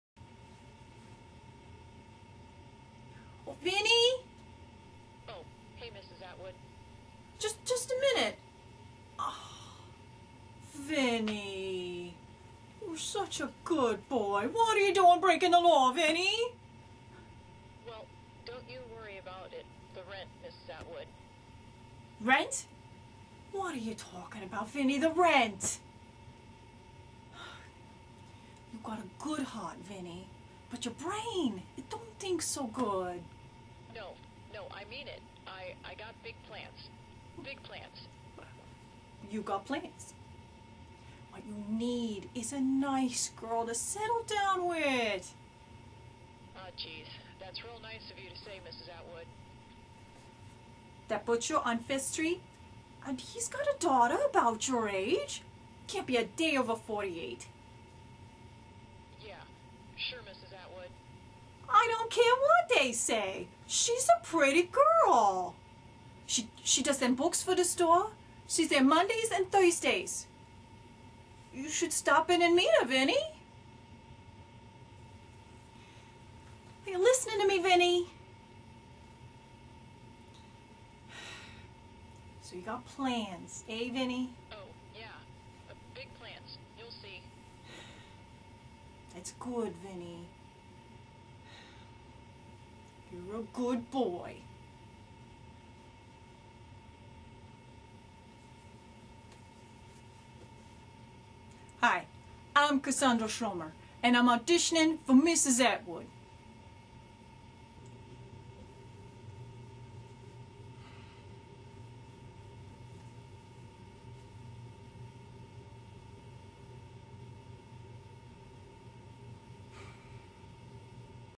Cassandra Schomer as Mrs. Atwood in curlers
Callbacks are great!  I was given the opportunity to audition for this role (Mrs. Atwood in "Clerical Errors") a second time, and was graciously given permission to post this video so my friends may see it.  My first version of Mrs. Atwood was quite the opposite of what you see here -- she was a very loud, animated, heavily make-upped woman.  Casting saw the first audition, liked my performance, and asked me to do this role again -- in a "ratty housecoat and with curlers in my hair".  Just to up the stakes a bit, I made her a little more "Bronx"-ish and fussy, just for fun.  Enjoy. :)